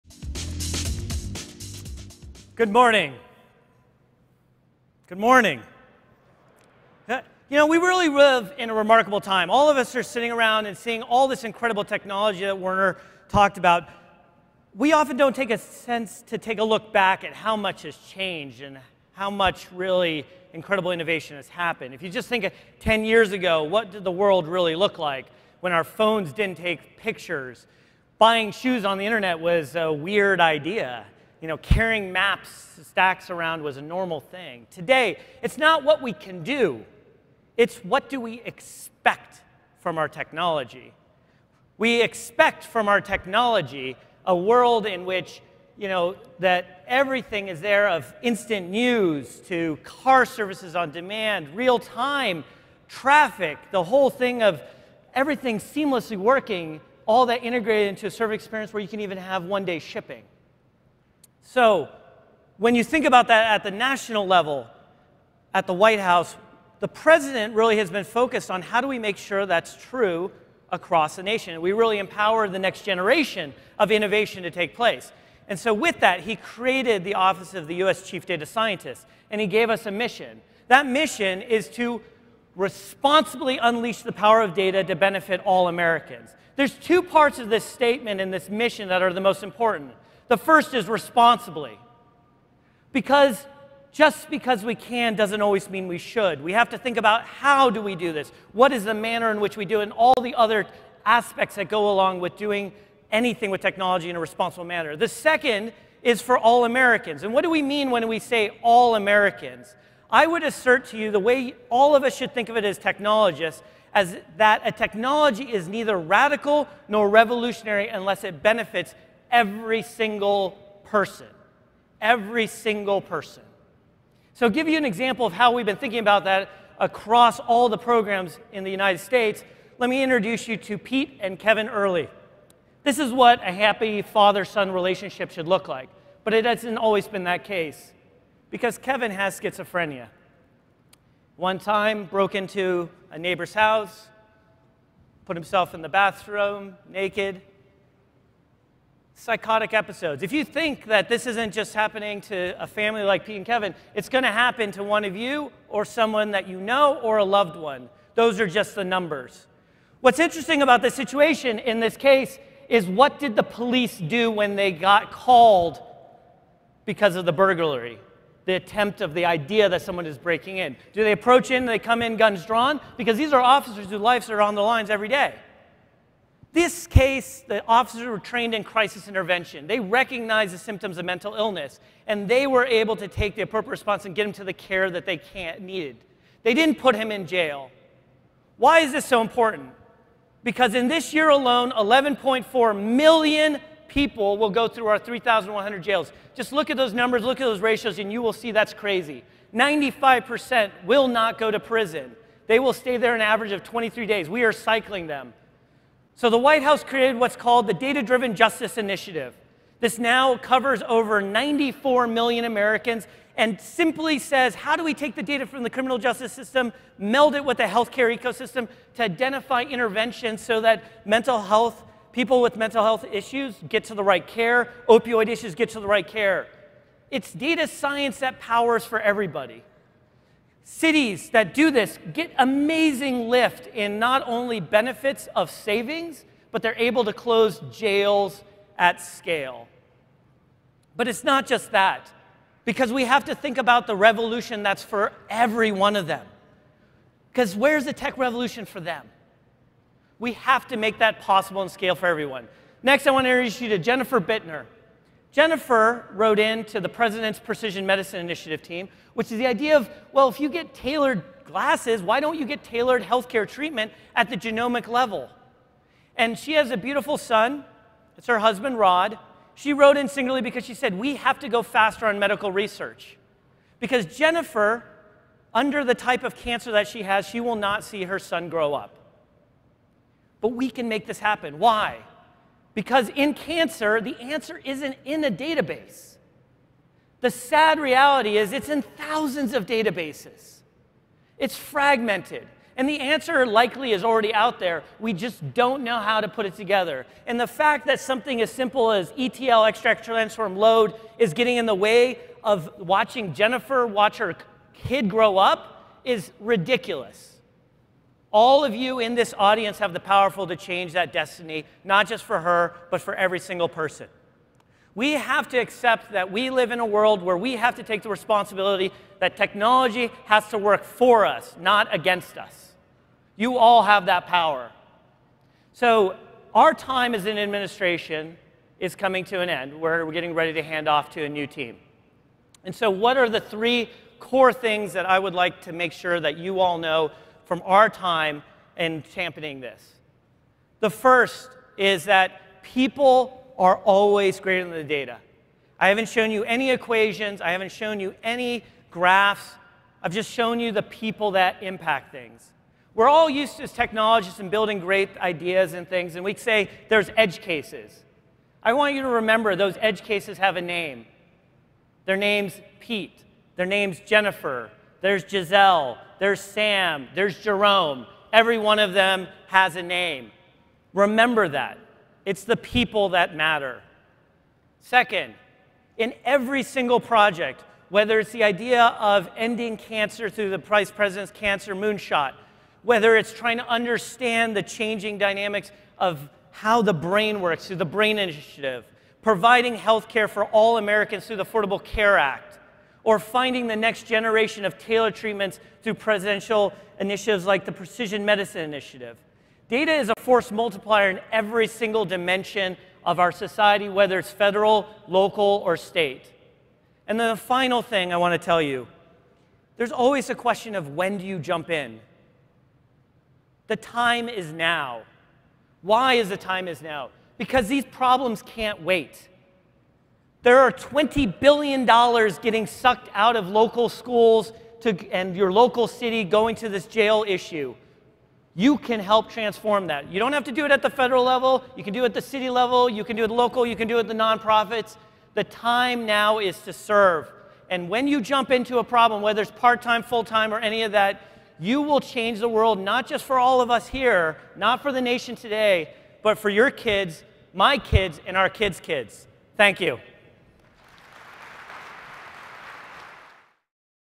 DJ Patil Shares How the White House Unleashes the Power of Data to Benefit All Americans
To learn more about how AWS can help meet your big data needs, visit our Big Data details

Through its Data-Driven Justice Initiative, the White House Office of Science and Technology Policy (OSTP) used data to provide mental health de-escalation training to police officers in Miami Dade, Florida, resulting in more than 10,000 people being diverting from jail and a savings of nearly $12 million per year. OSTP advises the President and others within the Executive Office of the President on the effects of science and technology on domestic and international affairs. The organization works on initiatives related to criminal justice, healthcare, science, higher education, and much more. Chief Data Scientist DJ Patil spoke onstage at re:Invent 2016.